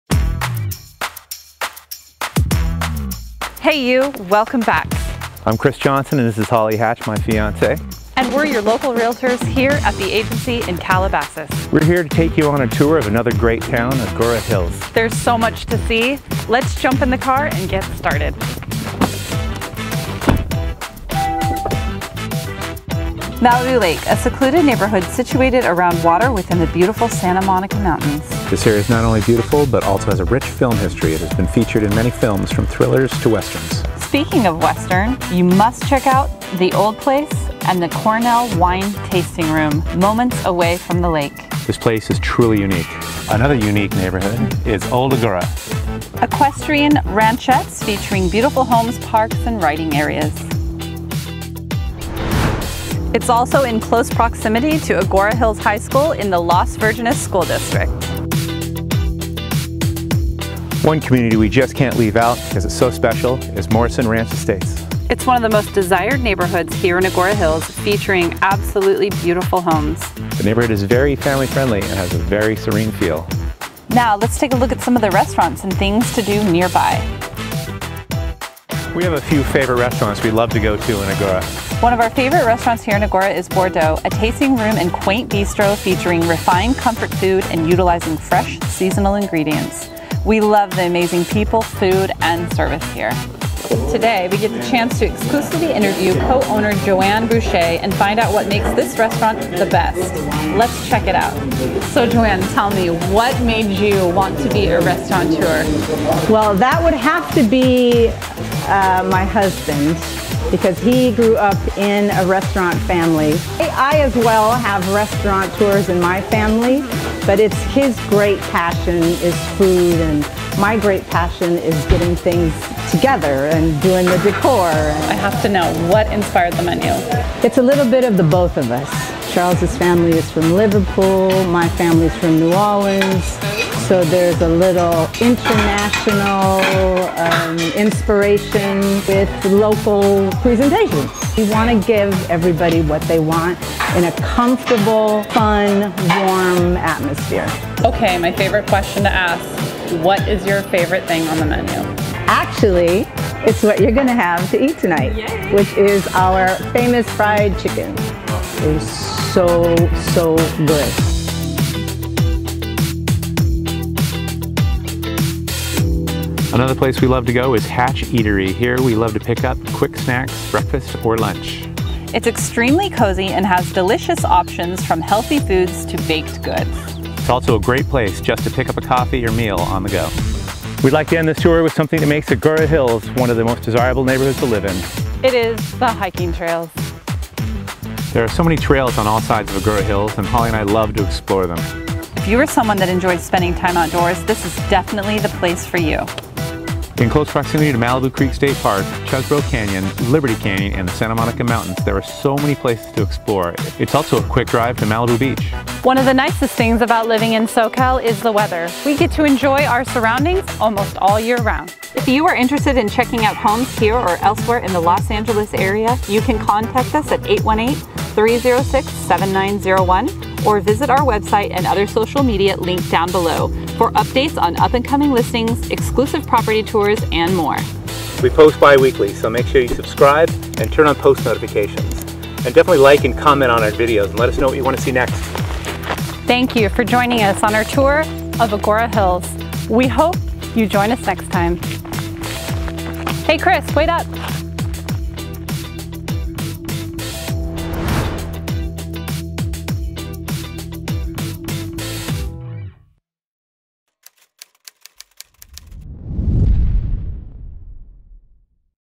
Agoura Hills Tour with Holly & Chris | The Agency
Join us on a quick walk-through of Agoura Hills, a unique lifestyle surrounded by lush landscape with a rural lifestyle. We visit a few locations like Malibou Lake and Cornell, where we find one of the best BBQ restaurants, The Old Place and the Cornell Wine tasting room. We speak with the owner of one of our favorite restaurants around, Boar Dough Tasting Room, and enjoy a delicious meal and a carefully curated wine list. Sitting between Calabasas and Westlake Village, Agoura Hills is more rural, with equestrian ranchettes and a rich Hollywood movie history.